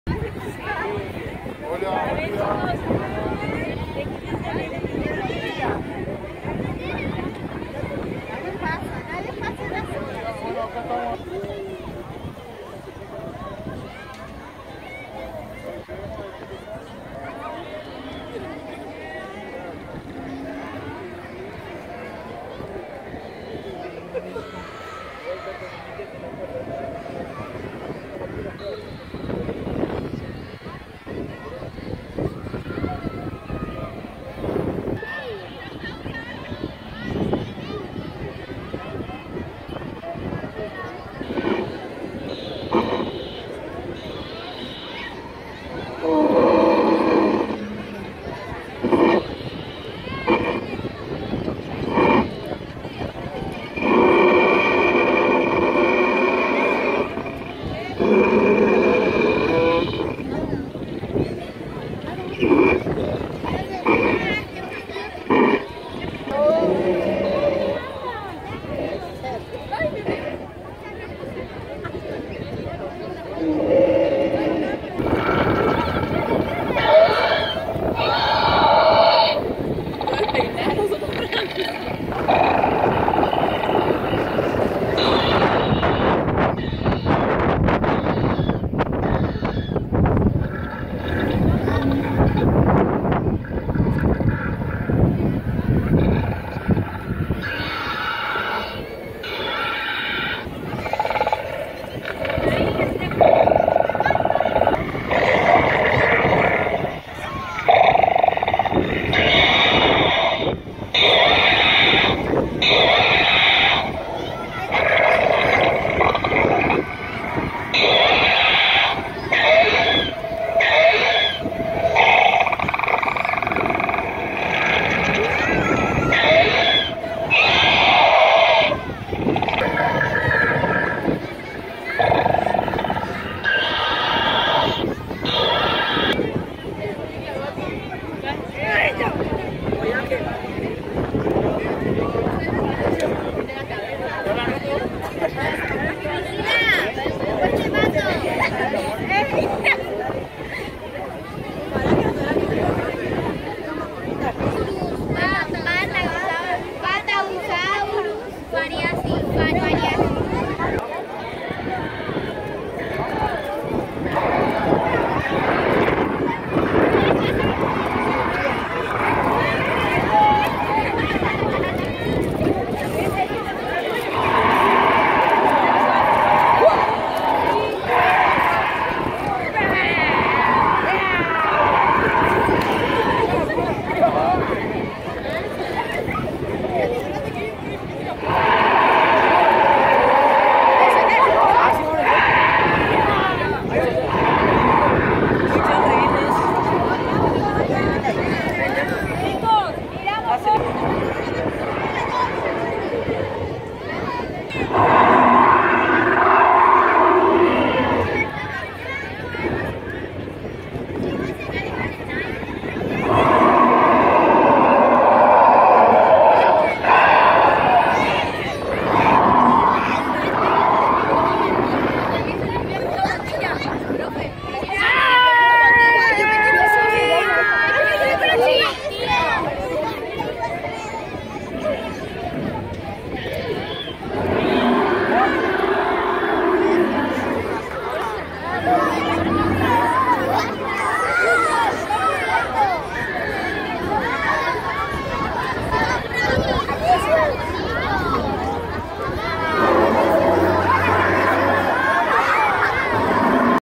Tiranosaurio / Tyrannosaurus Rex, Triceratops, Velociraptor en Tecnopolis Federal 2017
En este video presentamos al Kritosaurus, Protoceratops, Triceratops, Iguanodon, Velociraptor, Patagosaurus y el poderoso Tiranosaurio Rex.